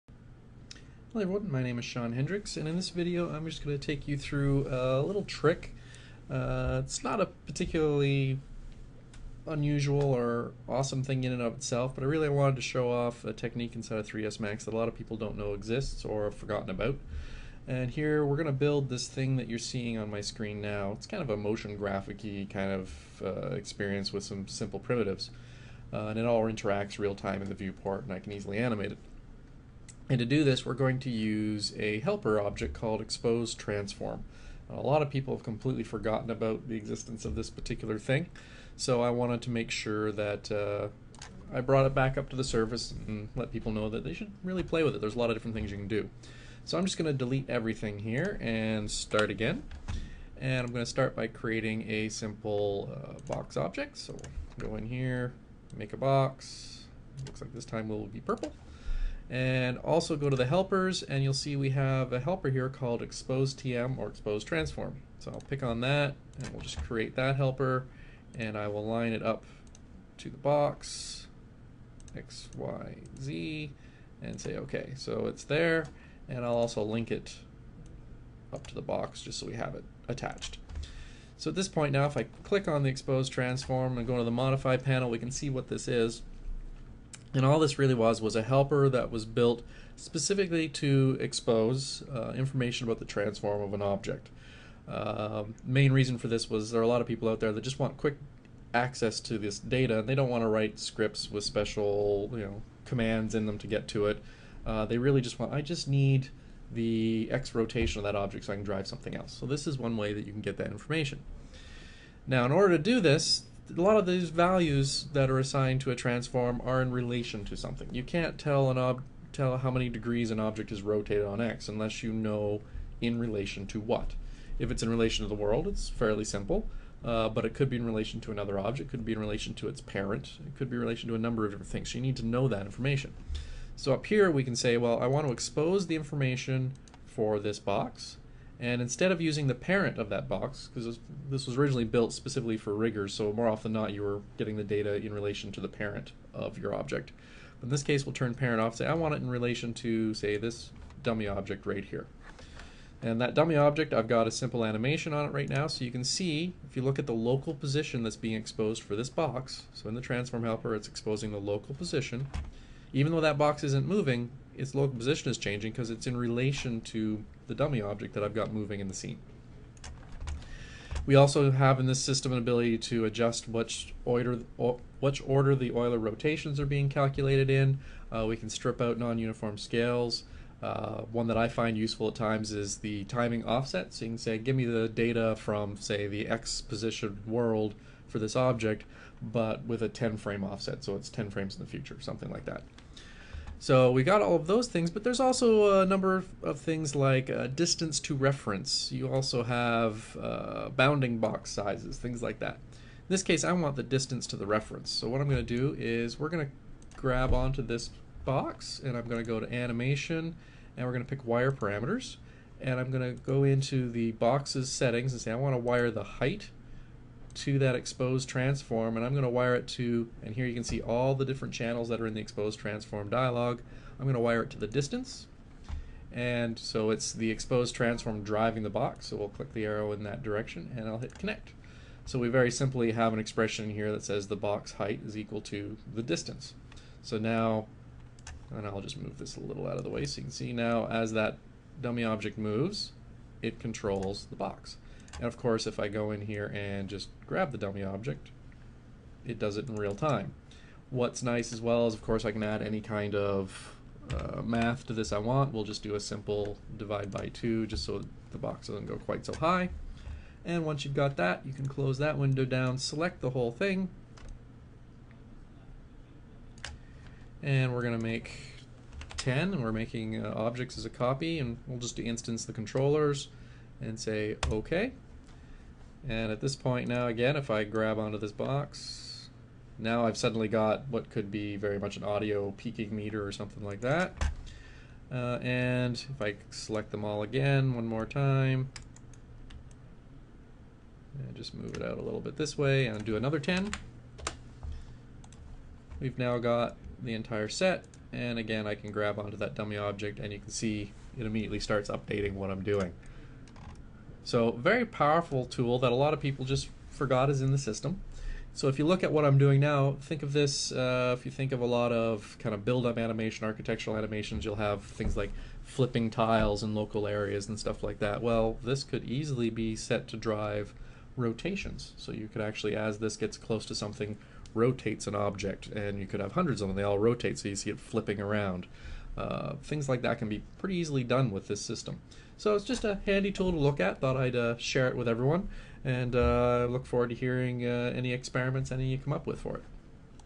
Expose Transform Trick for 3ds Max
In this video Shawn Hendriks explores the use of the expose transform helper in 3ds max to create quick motion graphic style animation